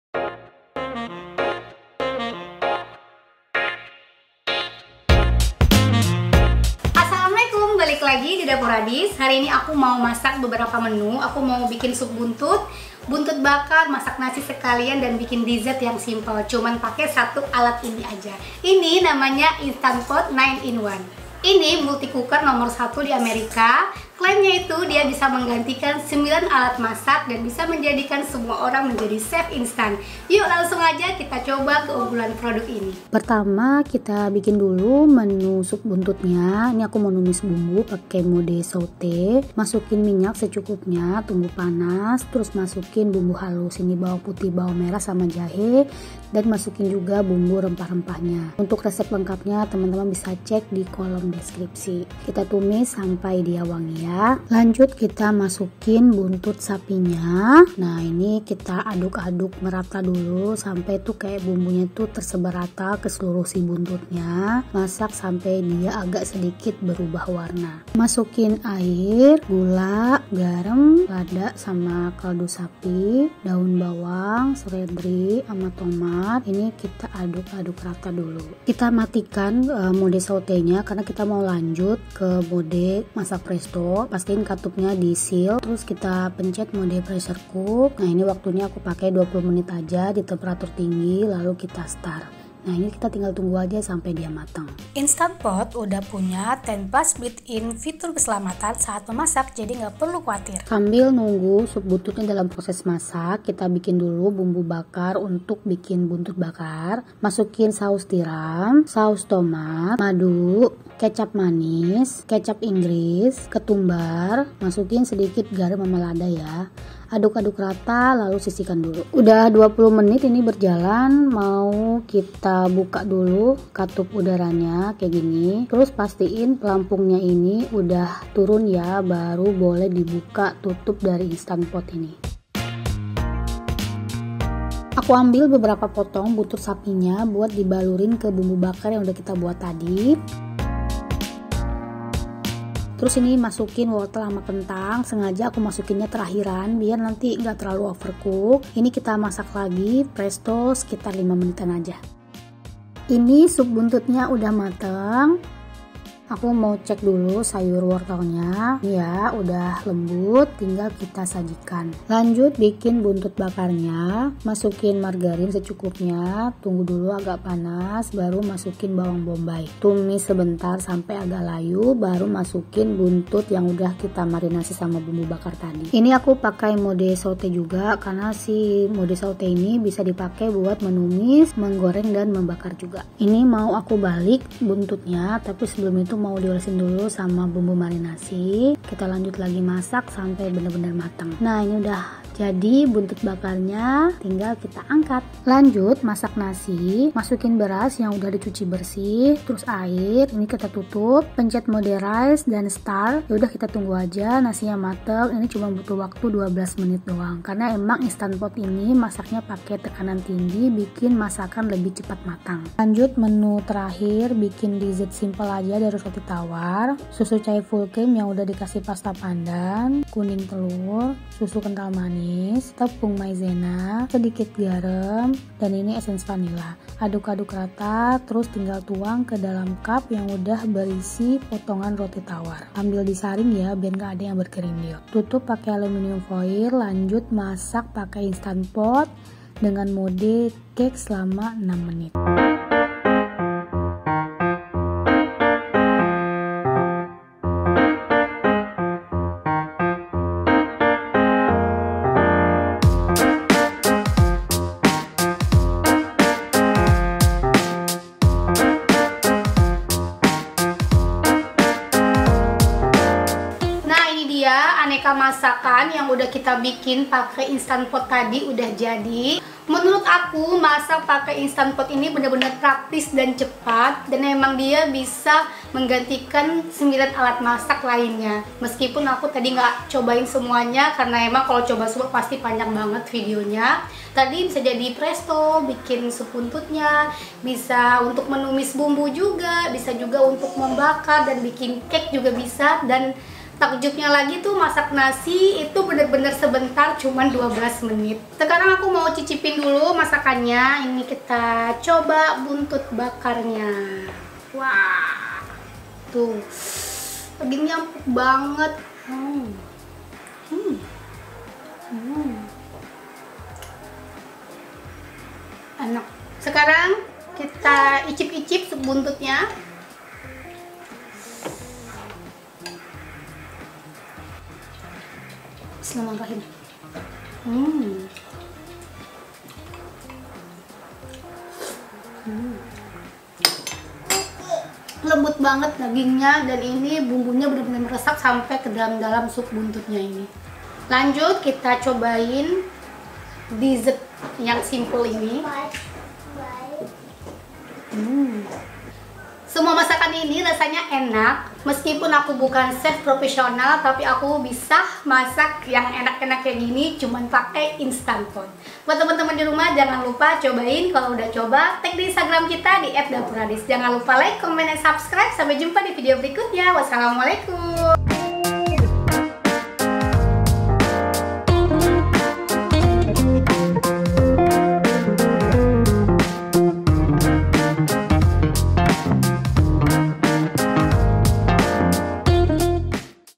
Honest Review Instant Pot! MASAK SOP BUNTUT, BUNTUR BAKAR, DESSERT HANYA PAKAI 1 ALAT INI!
Assalamu'alaikum..
Hai teman-teman jadi hari ini aku mau review Instant Pot yang memiliki keunggulan bisa menggantikan 9 alat masak! Masak nasi hanya butuh waktu 12 menit! Keren banget kan. Untuk lebih lengkapnya jangan lupa nonton video nya sampai selesai ya!

Resep Sup buntut

Bahan
1 kg buntut sapi
1 buah tomat
1 batang daun bawang
1 batang seledri
5 kapulaga
5 cengkeh
1 buah pala keprek
1 batang kayu manis
1,5 liter air
1/2 sdm garam
1/2 sdm gula
1 sdm kaldu sapi
1 sdt lada

Bumbu halus
7 siung bawang merah
5 siung bawang putih
1 ruas jahe

Pelengkap
Bawang goreng
Seledri
Jeruk nipis
Sambal

Resep Bumbu buntut bakar
1 sdm saus tiram
1 sdm saus tomat
1 sdm madu
3 sdm kecap manis
1 sdt kecap ingris
1 sdt ketumbar
Margarine
Bawang bombay

Dessert
150 susu uht
Pasta pandan secukupnya
2 kuning telur
40 gr SKM
Sejumput garan
1 sdm tepung meizena
1/2 sdt vanilla

Selamat mencoba di rumah teman-teman. Kalo udah recook, upload ke instagram dan tag kita di @dapuradis